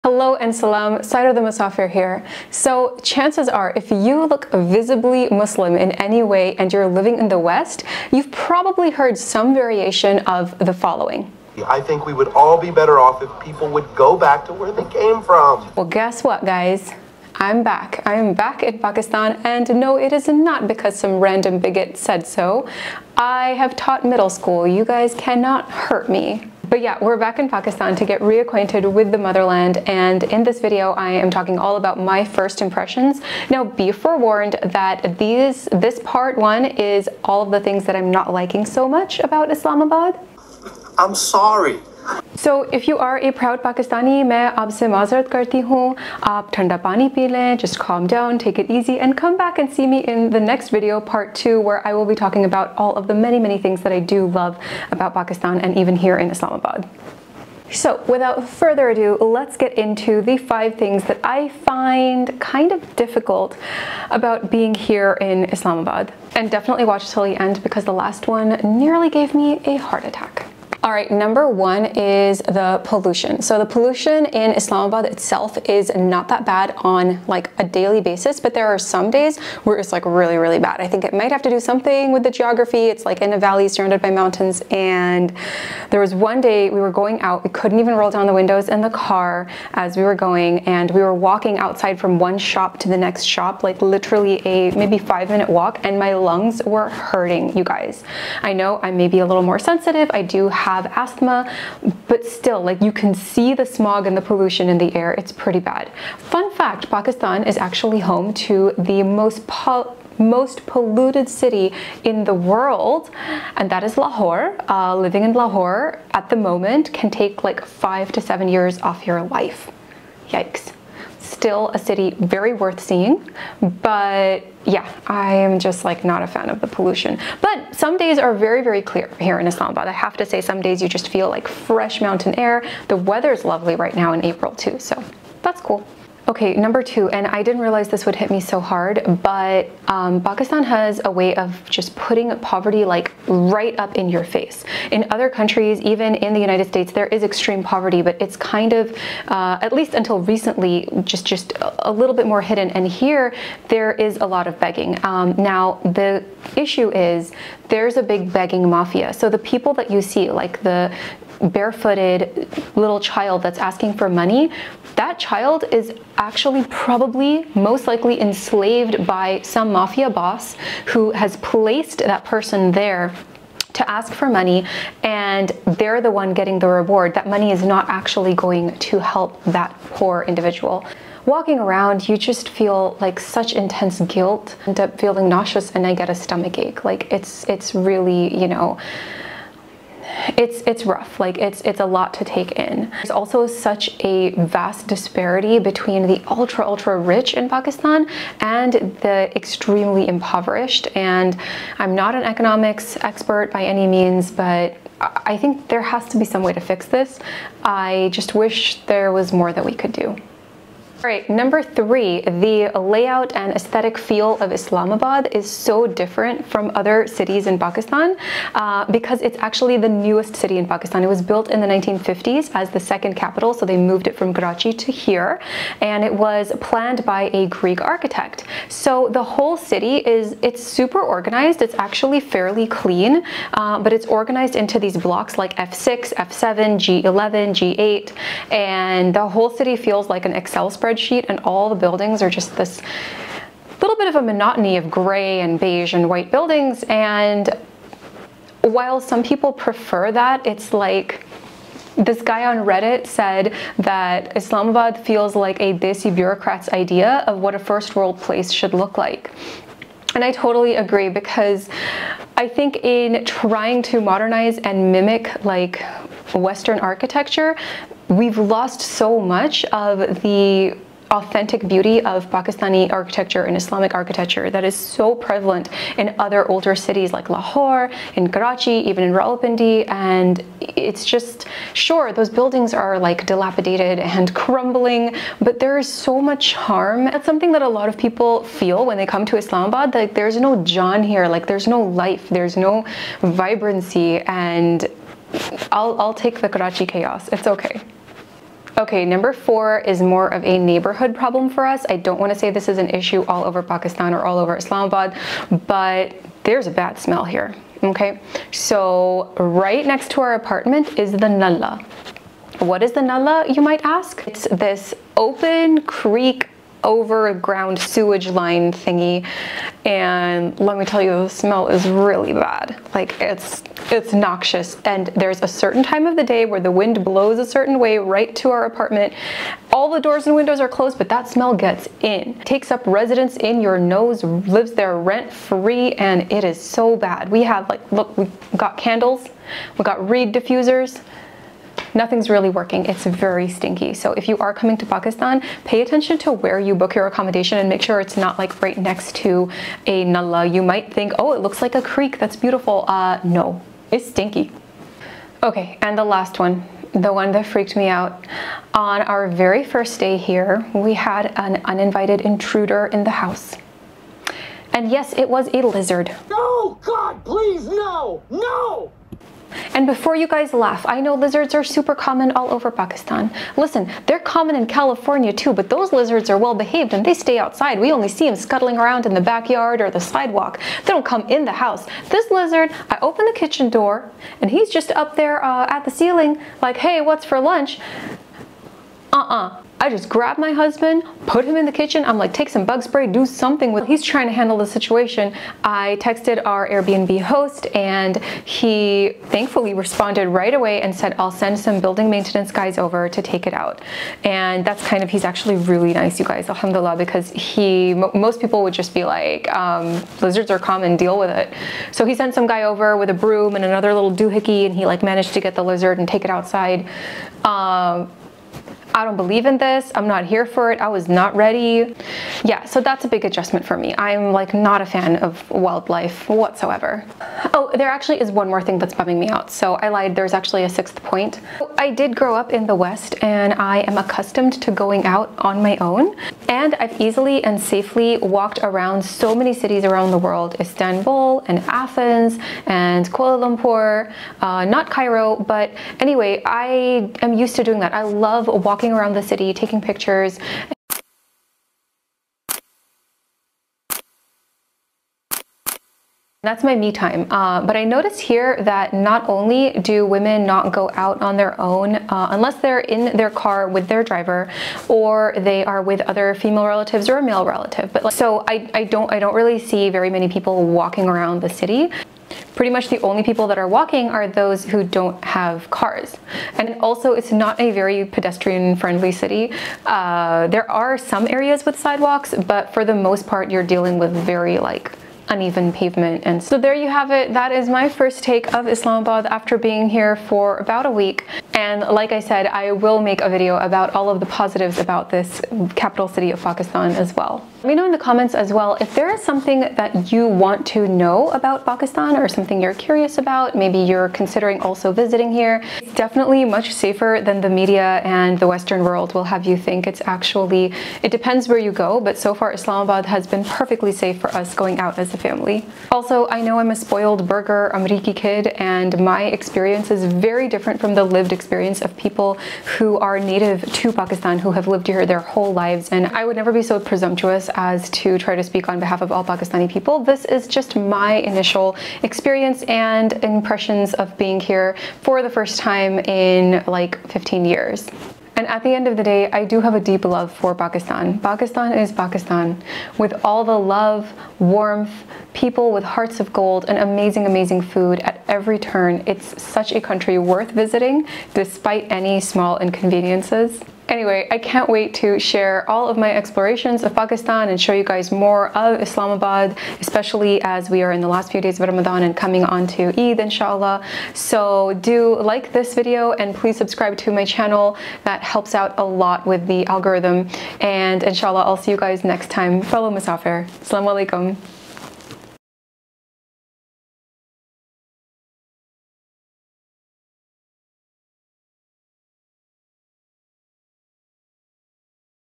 5 worst things about Islamabad, Pakistan | First Impressions Part 1
I’m in back Pakistan after 15 years! These are my first impressions of Islamabad. Part 1 is all about the things that I kinda don’t like about the capital city...sorry! Travel isn’t always as glamorous as it seems on social media, and I want to give you guys my honest opinion, so I’m sharing a few things that are bumming me out. Watch till the end for the moment that nearly gave me a heart attack.

That said, I do love so much about Pakistan, and Islamabad too, and I’ll be sharing that in PART TWO of this series.